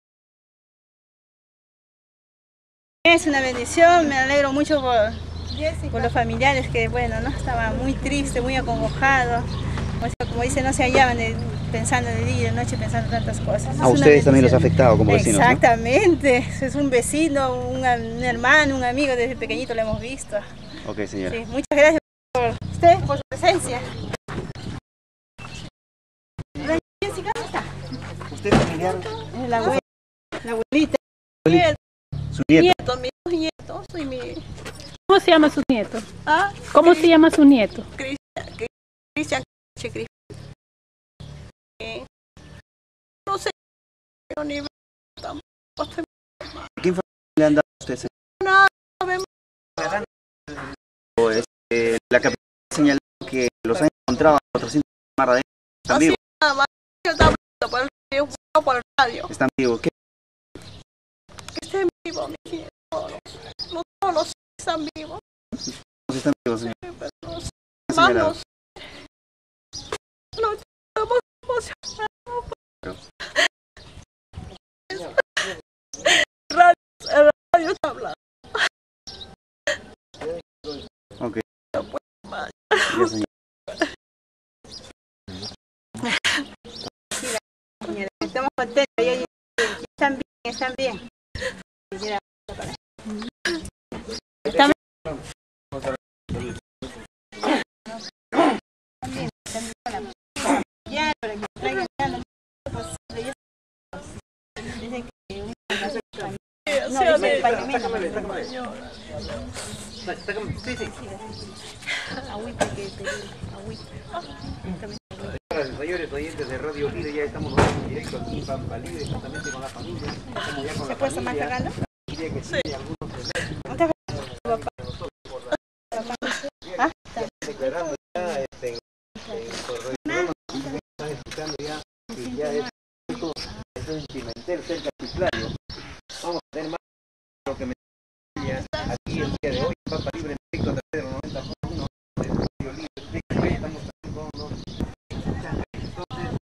Hallazgo de pescadores desaparecidos (parte2)
Abuela de Jhon y Oswaldo Quineche en su domicilio del Pueblo Joven Pampalibre